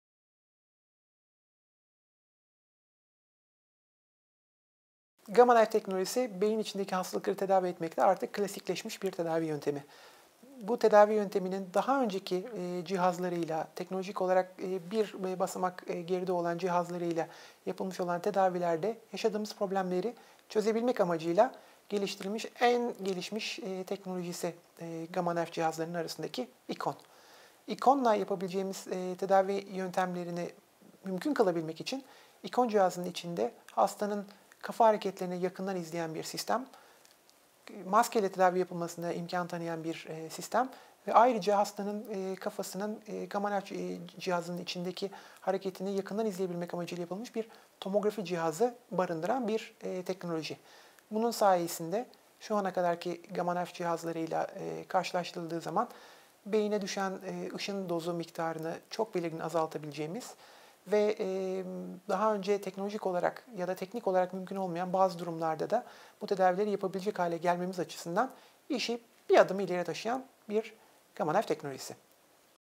Gamma Knife Icon'un Özellikleri Nelerdir?
Acıbadem Altunizade Hastanesi; Beyin ve Sinir Cerrahisi Uzmanı Prof. Dr. Koray Özduman; ""Gamma Knife Icon"" üzerine soruları cevaplıyor.
Acıbadem Sağlık Grubu'na desteklerinden dolayı teşekkür ederiz.